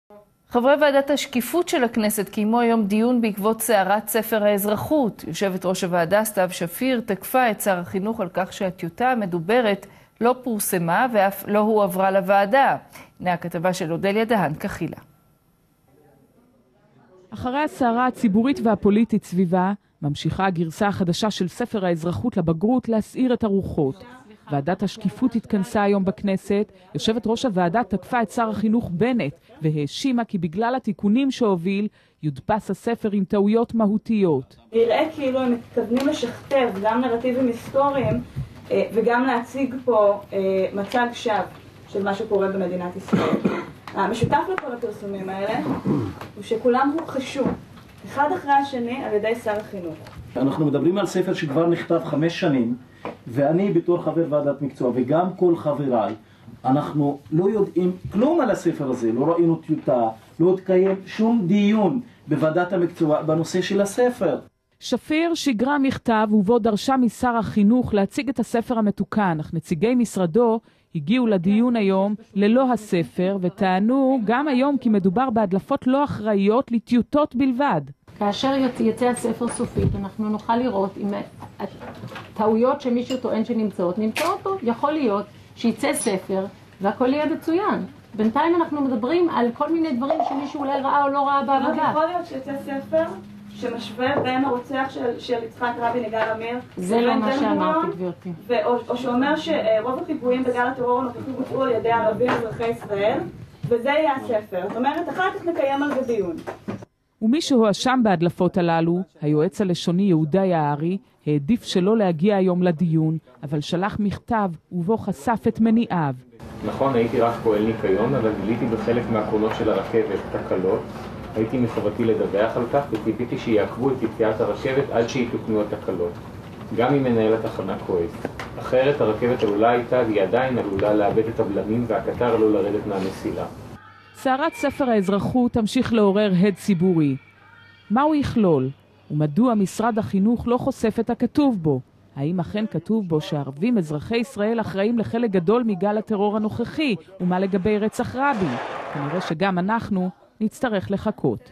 ערוץ הכנסת - סתיו שפיר על ספר האזרחות: נראה שמנסים לשכתב נרטיבים הסטוריים, 1.2.16
חברי ועדת השקיפות של הכנסת קיימו היום דיון בעקבות סערת ספר האזרחות. יושבת ראש הוועדה סתיו שפיר תקפה את שר החינוך על כך שהטיוטה המדוברת לא פורסמה ואף לא הועברה לוועדה. הכתבה של אודליה דהן כחילה.